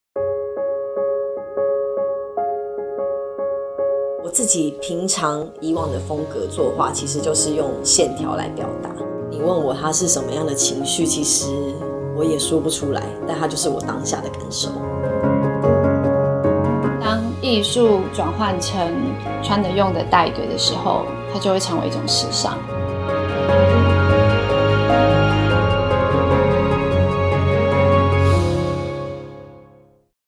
CHARLES & KEITH x BREEZE NIGHT 2018
CHARLES & KEITH launched two Taiwan-exclusive bags. Here’s a peek at the exclusive behind-the-scenes moments at the 2018 Breeze Night event in Taiwan.